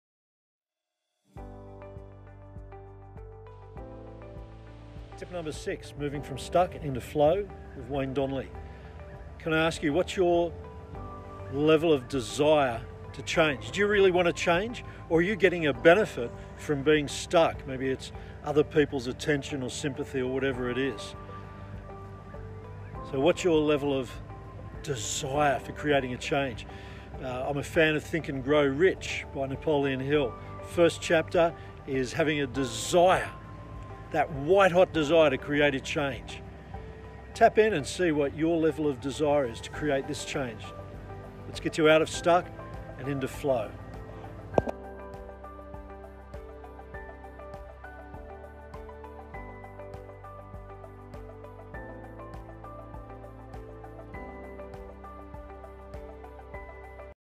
How hot is your Desire to change?
Desire is a powerful force to create change. In Think And Grow Rich, Napoleon Hill says your level of desire needs to be WHITE HOT!

How hot is your desire to change and get you out of being stick.

To learn more, contact Wayne